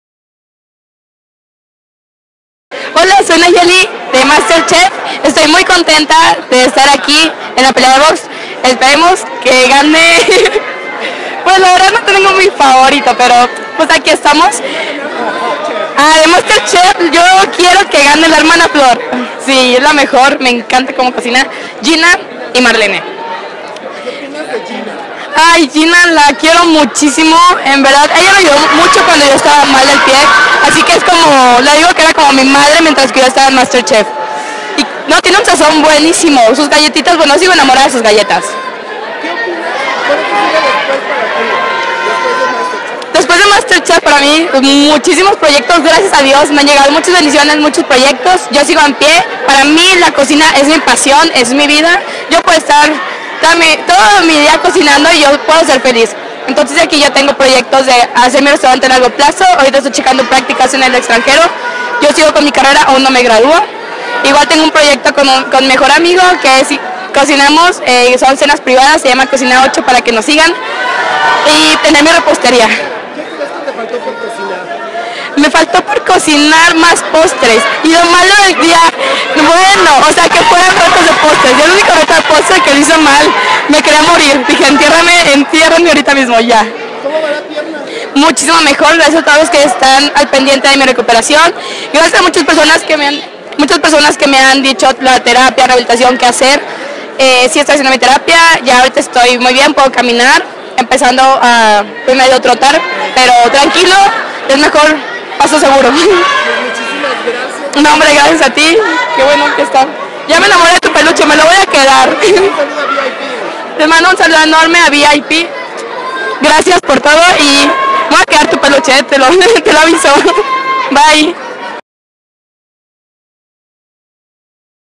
Vipink/Nayeli de Master Chef Mx
Nayeli de MasterCher nos dice su pronostico para la gran final. ¿Aquién ganará?